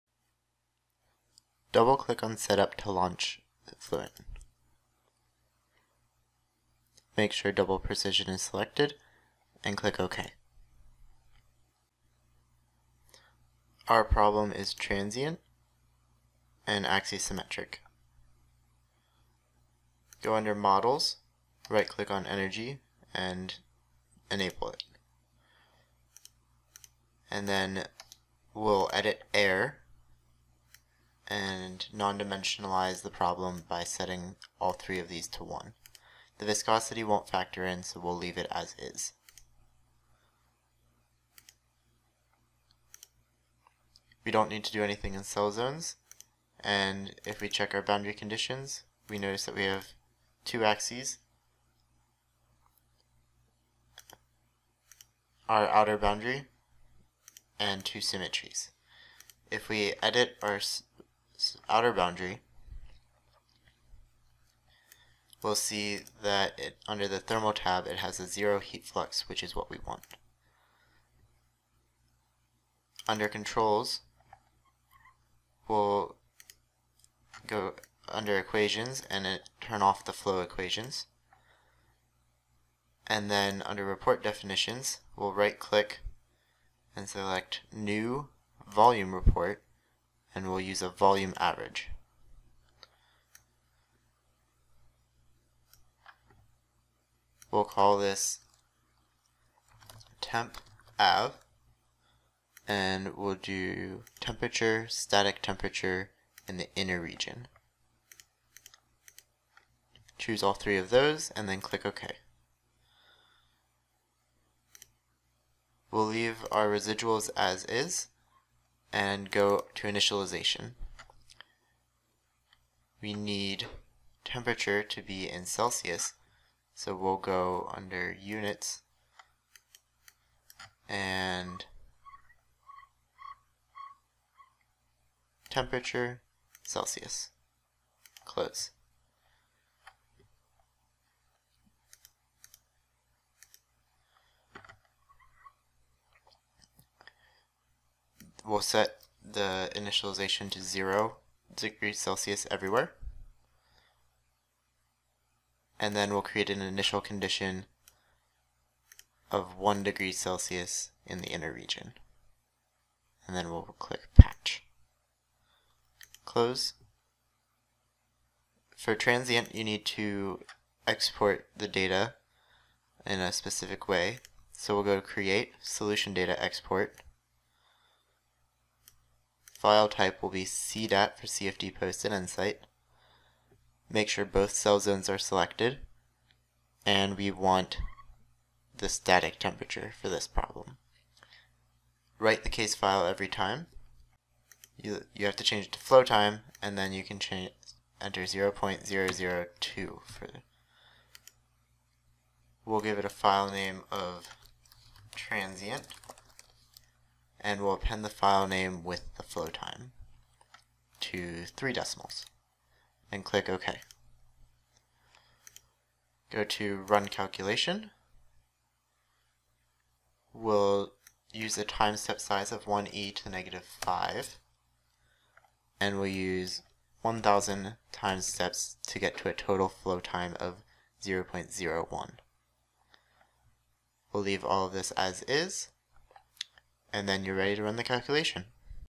2D Transient Diffusion: Model Setup
This video is from the “2D Transient Diffusion” tutorial from Cornell University at SimCafe.org